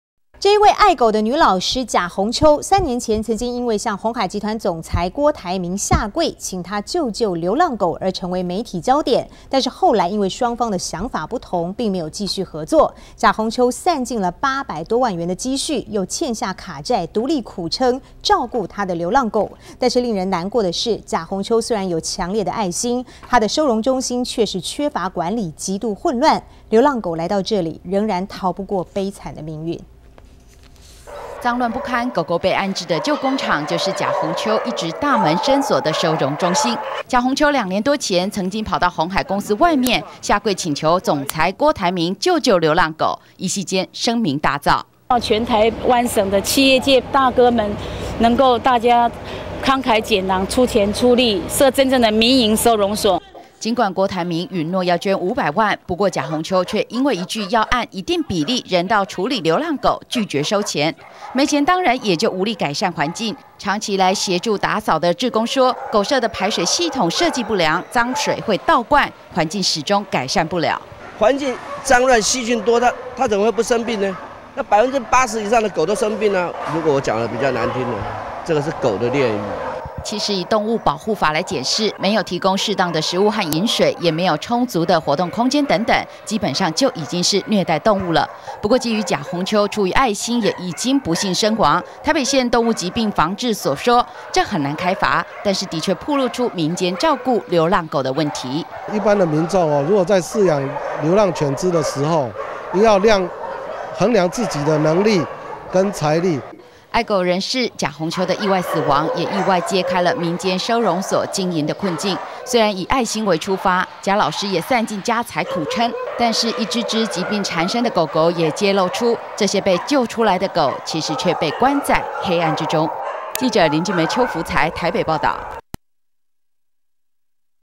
2010-02-22公視晚間新聞(賈鴻秋愛狗成痴 跪求郭台銘援手)
"這一位愛狗女老師--賈鴻秋三年前曾經因為向鴻海集團總裁--郭台銘下跪請他救救流浪狗而成為媒體焦點；但是後來因為雙方的想法不同後來並沒有繼續合作。賈鴻秋散盡八百多萬元的積蓄又欠下卡債獨力苦撐照顧她的流浪狗；但令人難過的是賈鴻秋雖然有強烈的愛心她的收容中心卻是缺乏管理極度混亂，流浪狗來到這裡仍然逃不過悲慘的命運。
髒亂不堪，狗狗被安置的舊工廠，就是賈鴻秋一直大門深鎖的收容中心，賈鴻秋兩年多前，曾經跑到鴻海公司外面，下跪請求總裁郭台銘，救救流浪狗，一夕間聲名大噪。
儘管郭台銘允諾要捐五百萬，不過賈鴻秋卻因為一句要按一定比例、人道處理流浪狗，拒絕收錢。沒錢當然也就無力改善環境，長期來協助打掃的志工說，狗舍的排水系統設計不良，髒水會倒灌，環境始終改善不了。
其實以「動物保護法」來檢視，沒有提供適當的食物和飲水，也沒有充足的活動空間等等，基本上就已經達到了虐待動物的程度。不過基於賈鴻秋出於愛心，也已經不幸身亡，台北縣動物疾病防治所說，很難開罰，但是的確暴露出民間照顧流浪狗的問題。
愛狗人士賈鴻秋的意外死亡，也意外揭開了民間收容所經營的困境，雖然以愛心為出發，賈老師也散盡家財苦撐，但是一隻隻疾病纏身的狗狗，也揭露出，這些被救來的狗，其實卻被關在黑暗之中。
記者林靜梅邱福財台北報導。"